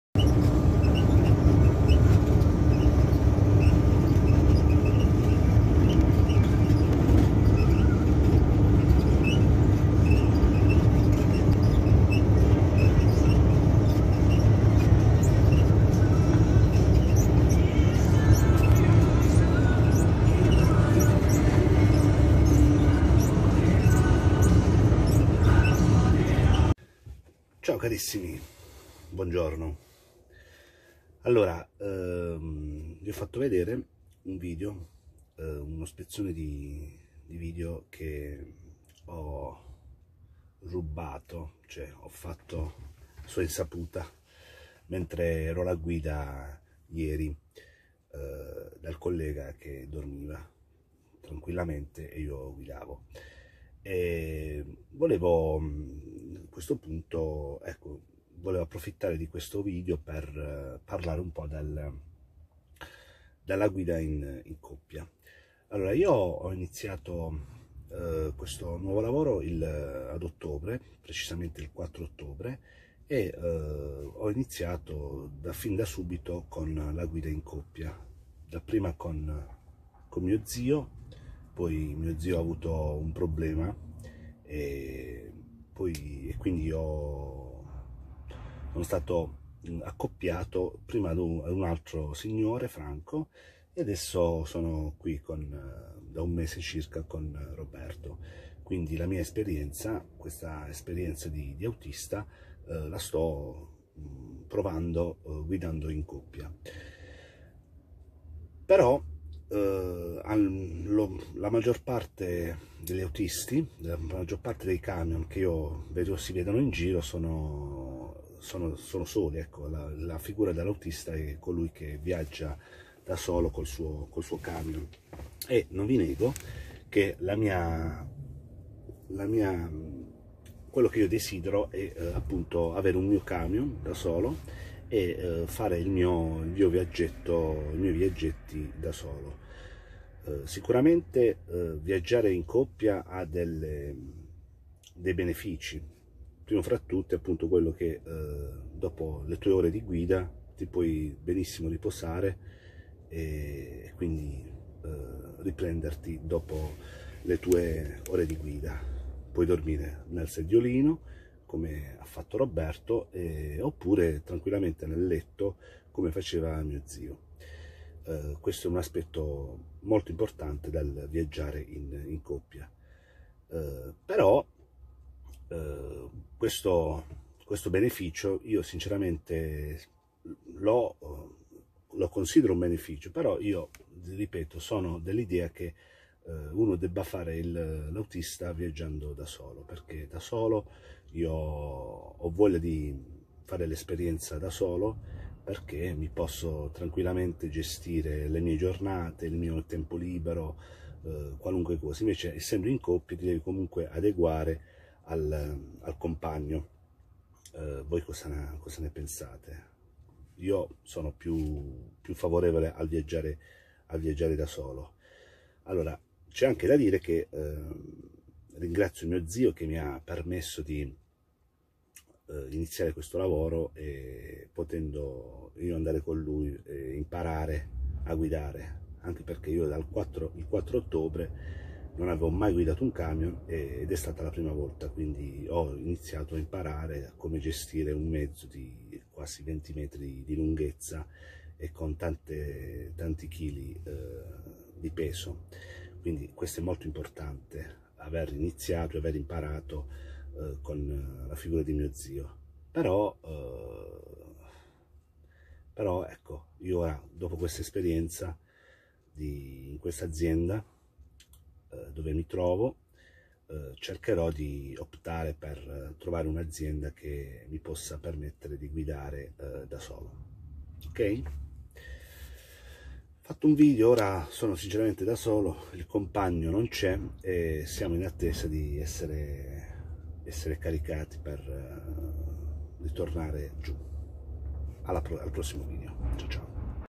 Il doppio autista
Ho intrapreso questo lavoro perché, grazie a mio zio, ho potuto imparare, fin da subito, come saper guidare un bestione da tante tonnellate e con quasi 20 metri di lunghezza.
La mia esperienza quindi da autista è, per il momento, solo con la guida in coppia. Sono dell'idea però che il vero autista è colui che guida solo con il suo mezzo, con le dovute limitazioni che una guida in coppia permette di superare.
Per il momento "mi accontento" del compagno di viaggio ma il mio obiettivo e poter un giorno guidare da solo.